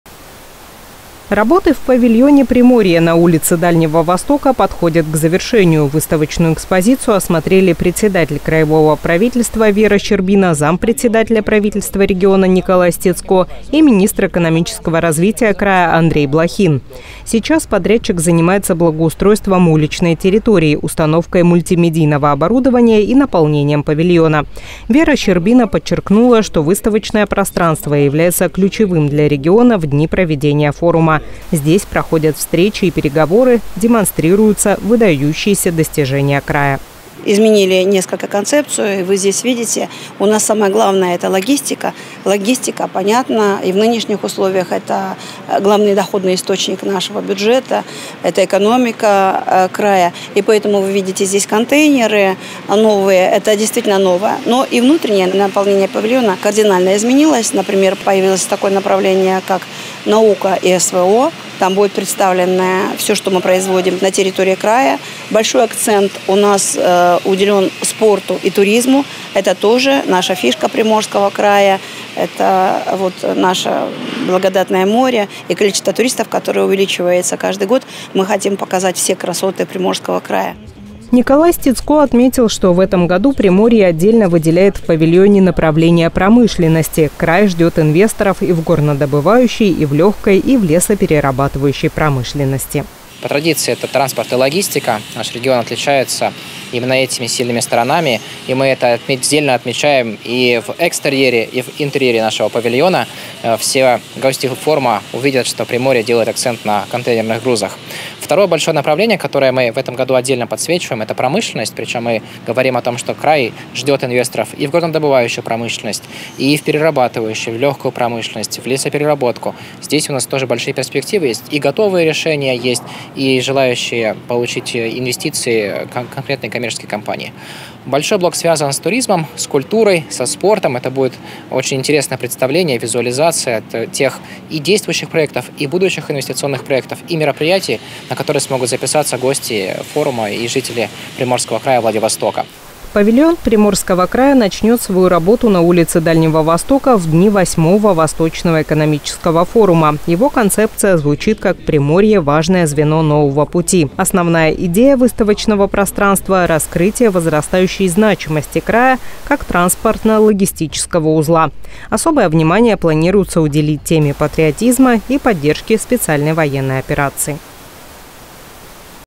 Вера Щербина проверила готовность павильона Приморья к приему участников ВЭФ-2023.
Работы в павильоне Приморья на Улице Дальнего Востока подходят к завершению. Выставочную экспозицию осмотрели председатель краевого правительства Вера Щербина, зампредседателя правительства региона Николай Стецко и министр экономического развития края Андрей Блохин. Сейчас подрядчик занимается благоустройством уличной территории, установкой мультимедийного оборудования и наполнением павильона.